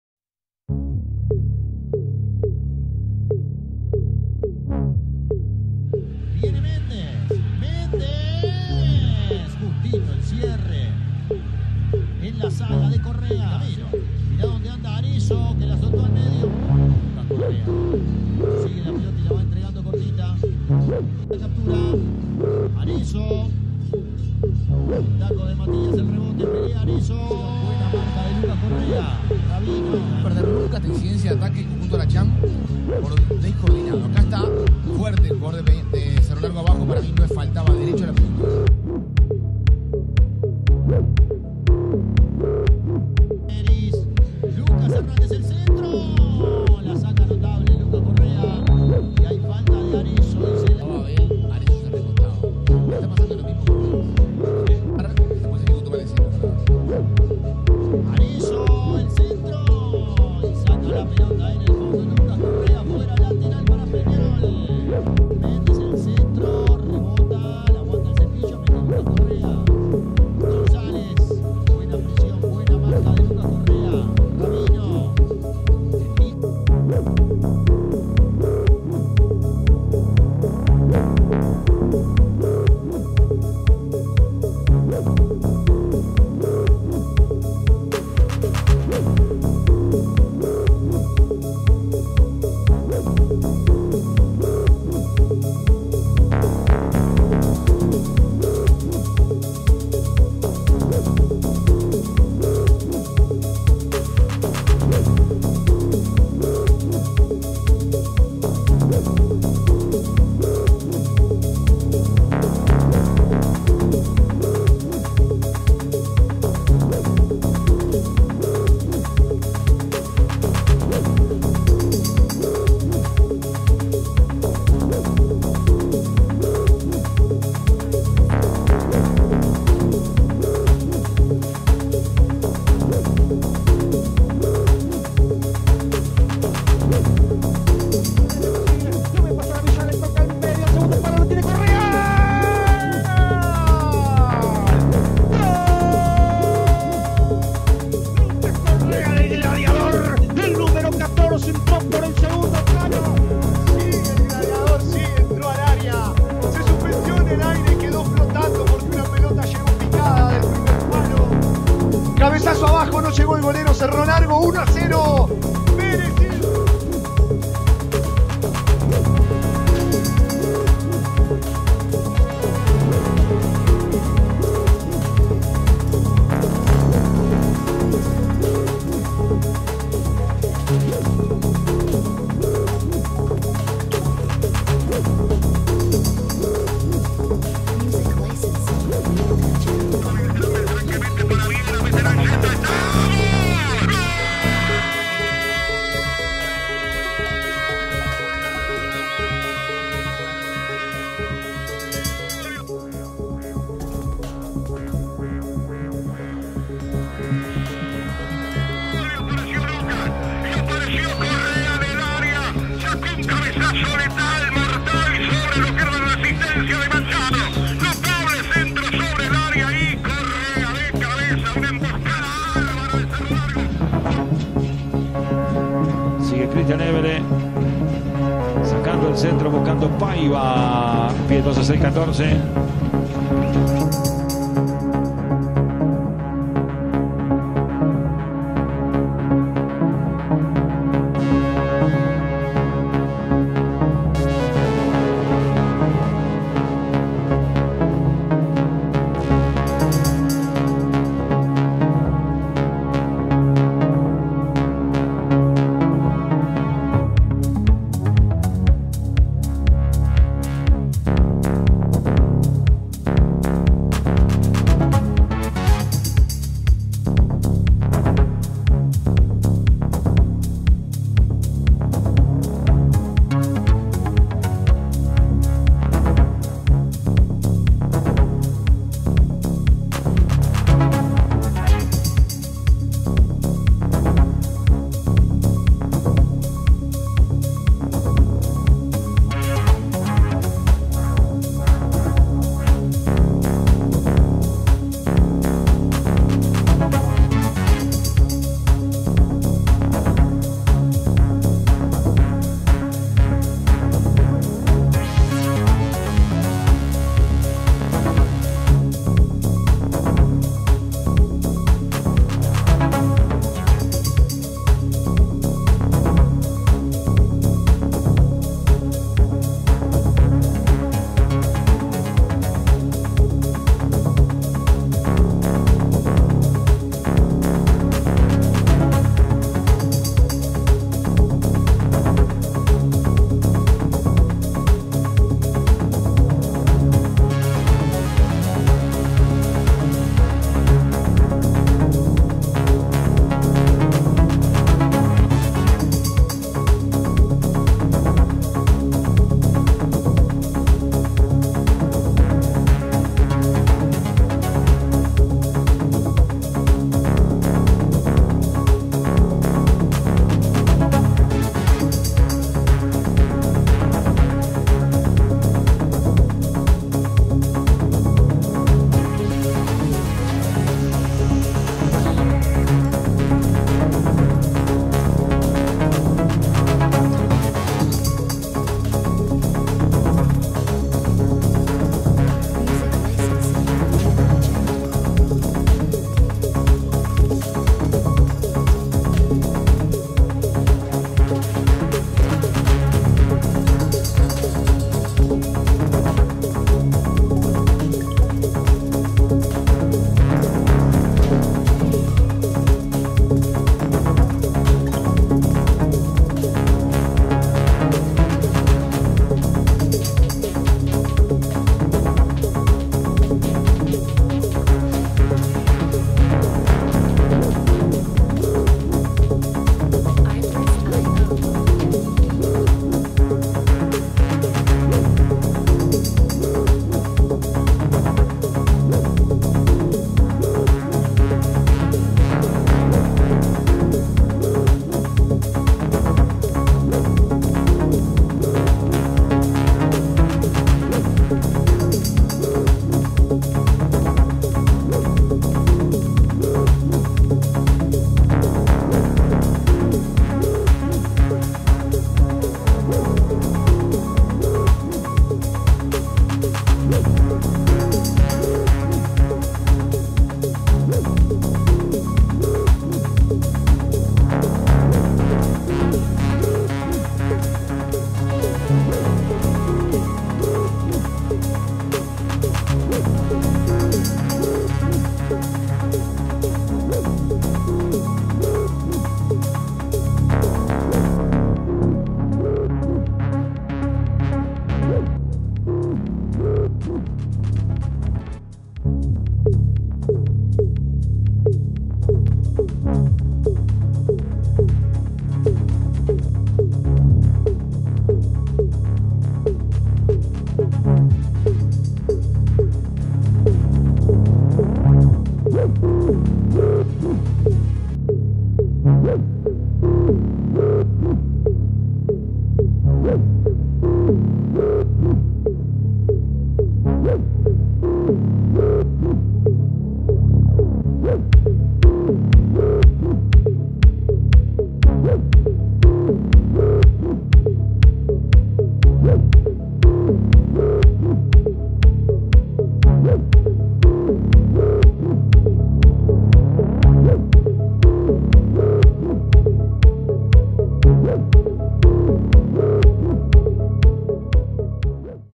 Lucas Correa Sebben 2023 HighLights, Defensive Skills, Tackles & Goals. Cerro Largo FC
● Lucas Leonel Correa Sebben ●

 ¤ Nacionality: Urugay  // Italy   🇺🇾//🇮🇹

☆ Defensive Skills, Tackles & Goals ☆

◇ Cerro Largo FC Season 2023

Manie HD